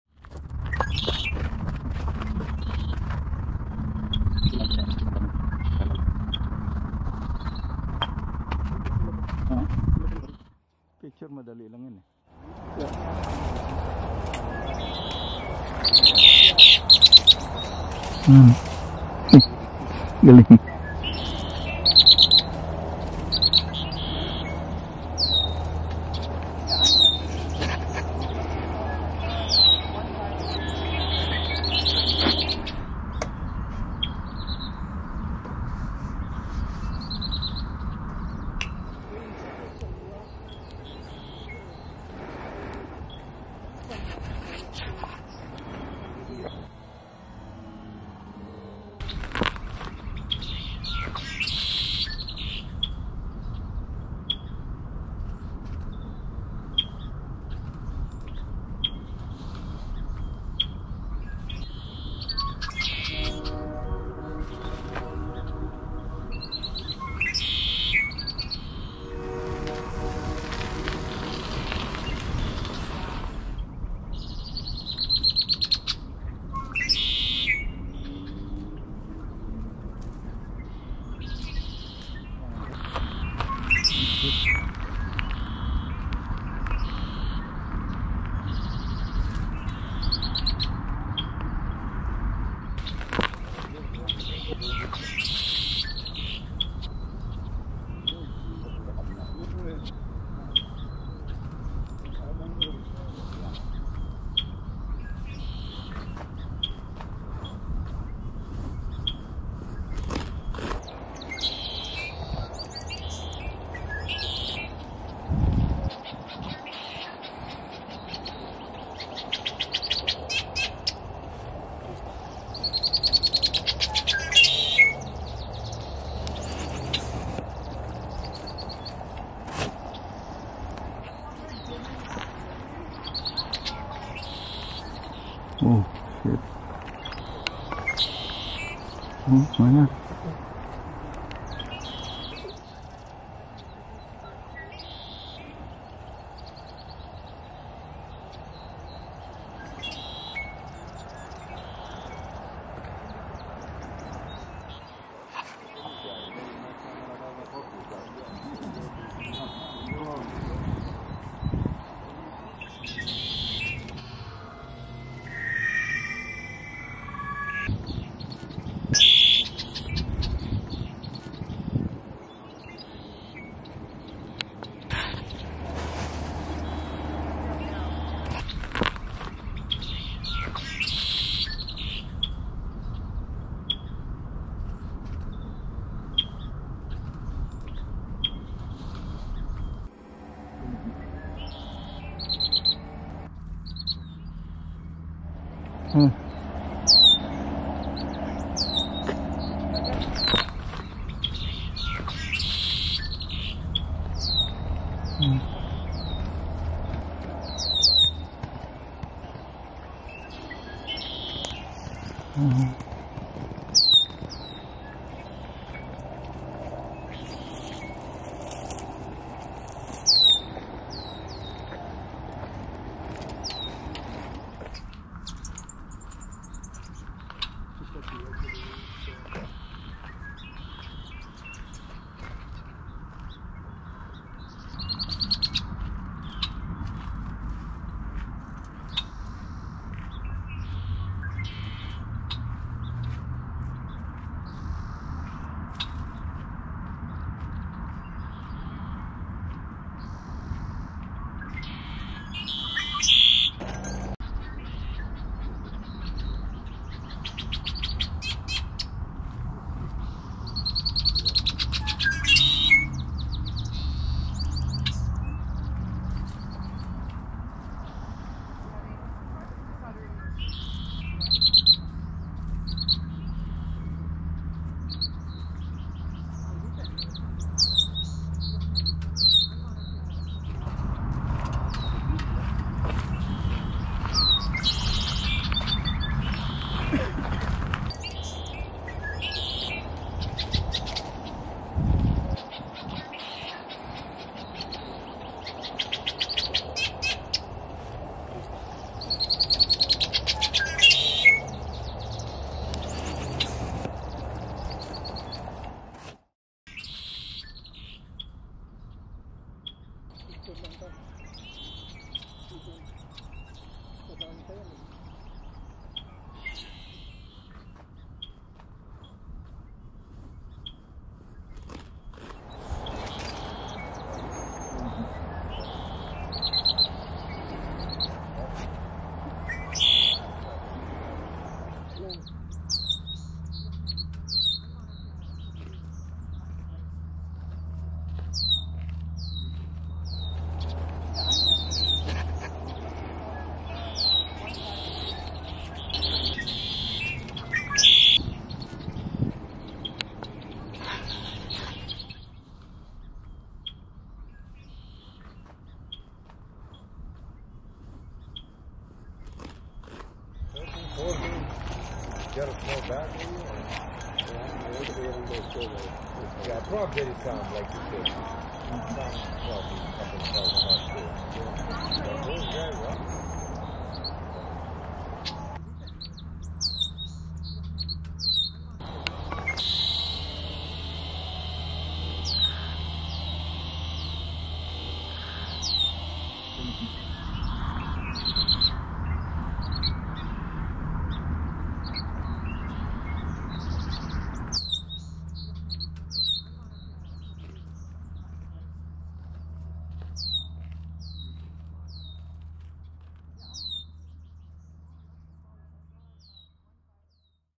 Colony Farm Regional Park | Birds Habitat | Bird Watching | Port Coquitlam BC, Canada
Bird Watching along Traboulay Poco Trail at Colony Farm Regional Park | Birds Habitat | Port Coquitlam BC, Canada